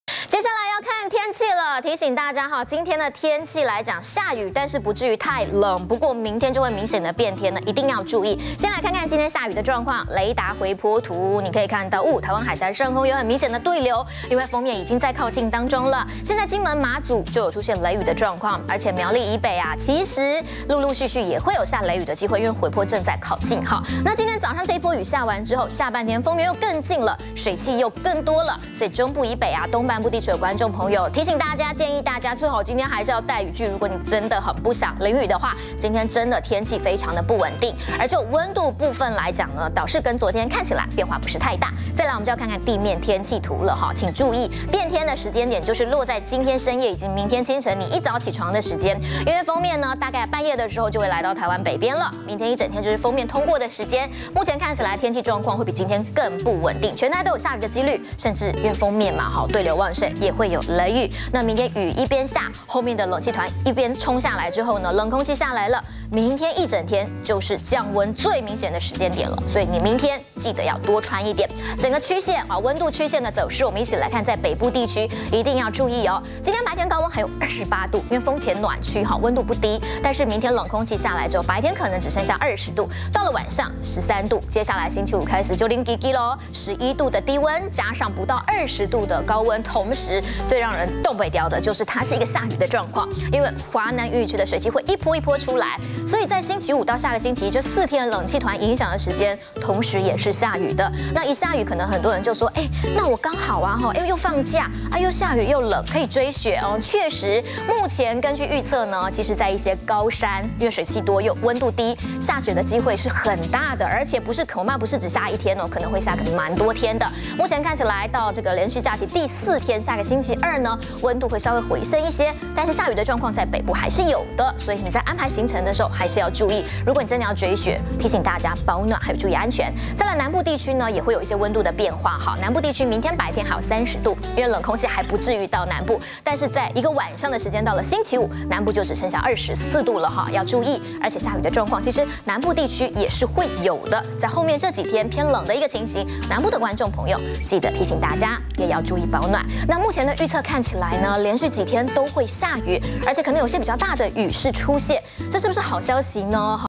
20170222中天新聞 【氣象】鋒面近對流旺 苗栗以北沿海將雷雨
外島金馬已有雷雨，交通多注意安全。今下半天天氣更不穩，帶雨具防雷雨。今中部以北、東部，降雨機率都偏高。今天清晨比較涼，最低溫新屋14.5度。今北東高溫27-28度，中南部28-30度。鋒前暖濕，今明金馬易有低雲局部霧。今深夜鋒面到，明天一早北部就變天。明鋒面過＋冷氣團南下，一天轉濕冷。明天天氣糟！鋒面通過北東易有雷雨。明天氣溫溜滑梯，晚間北東只剩13度。冷氣團加華南水氣，高山降雪機會高。